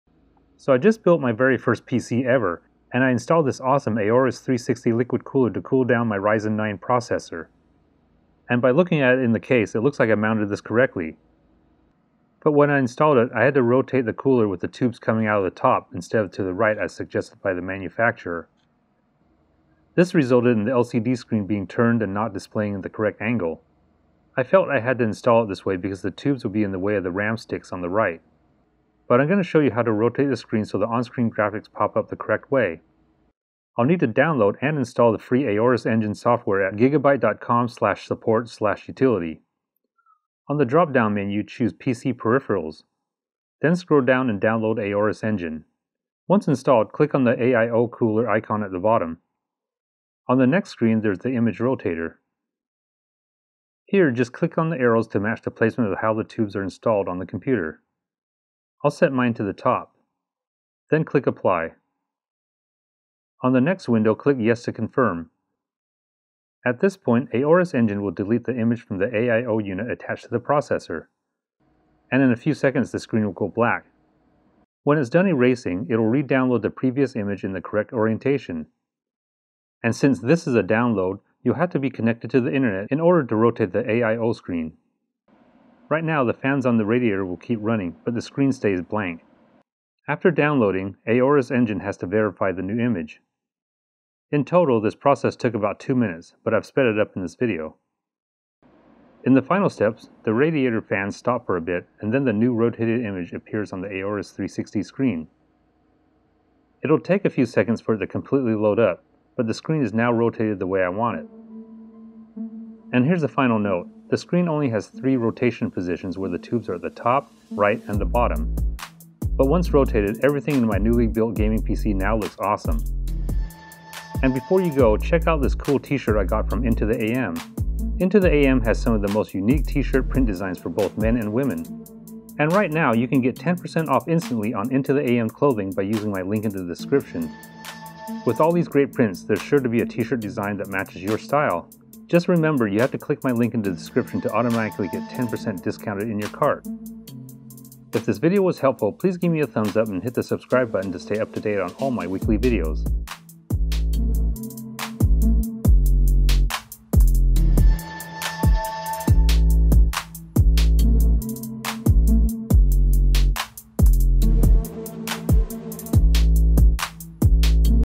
Aorus 360 Rotate LCD Screen - Aorus 360 Liquid Cooler AIO How to Rotate Screen After Installation
Aorus 360 Liquid CPU Cooler
CPU Thermal Paste

When I built my very first PC and installed an Aorus 360 Liquid Cooler, I had to rotate the cooler with the tubes coming out at the top instead of to the right as suggested by the manufacturer. This resulted in the LCD screen being turned and not displaying at the correct angle. I felt I had to install it this way because the tubes would be in the way of the RAM sticks on the right. But in this video I'm going to show you how to rotate the screen so the on-screen graphics pop up the correct way.

First thing to do is download and install the free Aorus Engine software On the dropdown menu choose PC Peripherals. Then scroll down and download Aorus Engine. Once installed, click on the AIO cooler icon at the bottom.

On the next screen there's the Image Rotator. Just click on the arrows to match the placement of how the tubes are installed on the computer. I'll set mine to the top. Then click Apply. On the next window click Yes to confirm. At this point, Aorus Engine will delete the image from the AIO unit attached to the processor. And in a few seconds the screen will go black. When it's done erasing, it'll re-download the previous image in the correct orientation. And since this is a download you'll have to be connected to the internet in order to rotate the AIO screen. Right now the fans on the radiator will keep running but the screen stays blank. After downloading, Aorus Engine has to verify the new image.

In total, this process takes about 2 minutes. In the final steps the radiator fans stop for a bit and then the new rotated image appears on the Aorus 360 screen. It'll take a few seconds for it to completely load up but the screen is now rotated the way I want it. And here's a final note, the screen only has 3 rotation positions where the tubes are on the top, right and the bottom. But once rotated, everything in my newly built gaming PC now looks awesome!